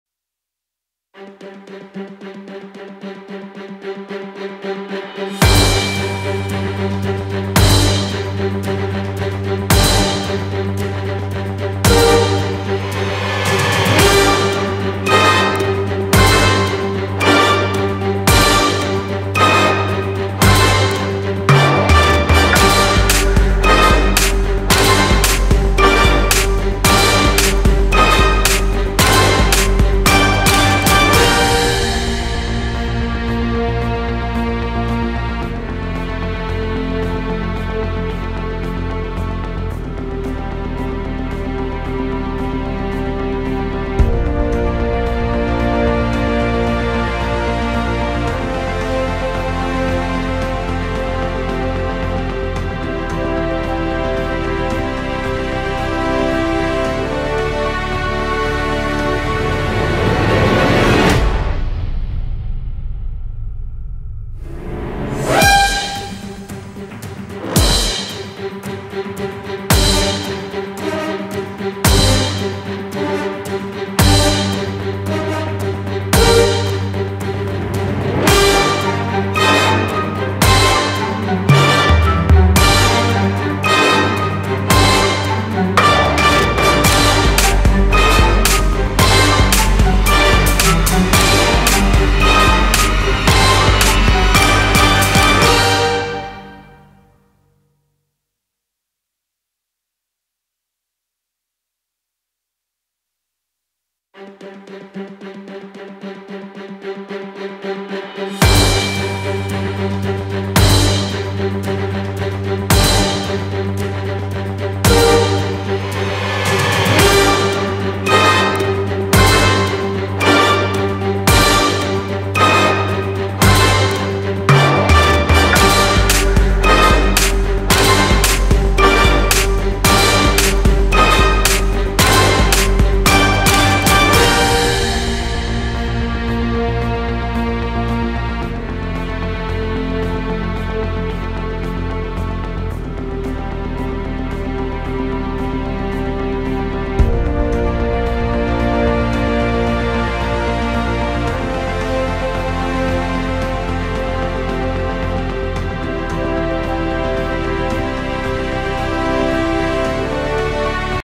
Ipad Painting: Bumblebee timelapse
The new trailer for the Bumblebee movie came out this week and i just got really hyped!
Did a quick painting study :) will probably do Miles Morales next

Painted in ipad pro 2nd gen
software used : Procreate